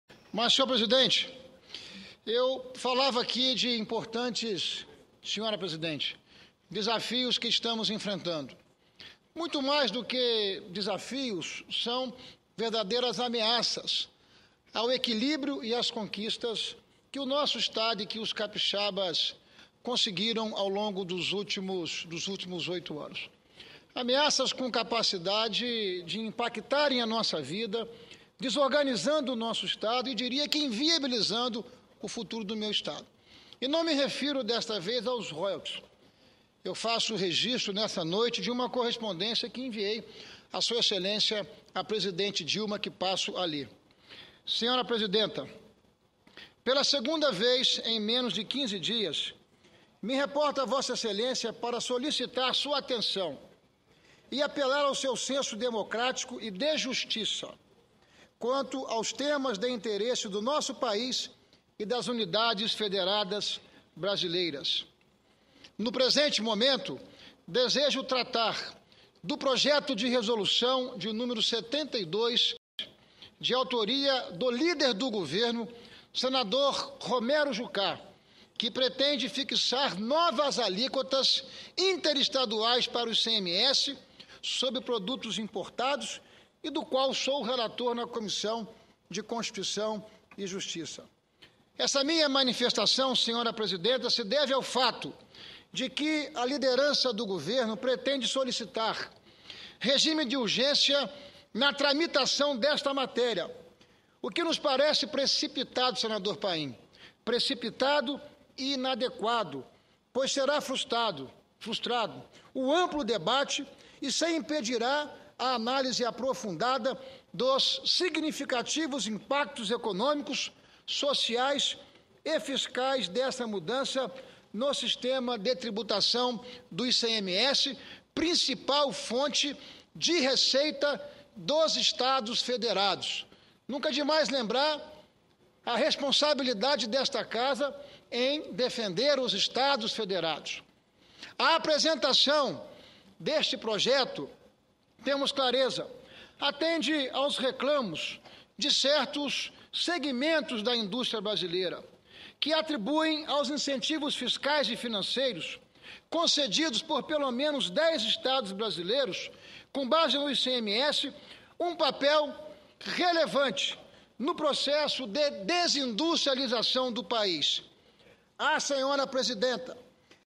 Sen. Ricardo Ferraço, PMDB/ES, envia nova carta a Dilma para falar da situação dos estados
30/11/2011 - Plenário - Sen. Ricardo Ferraço, PMDB/ES, envia nova carta a Dilma para falar da situação dos estados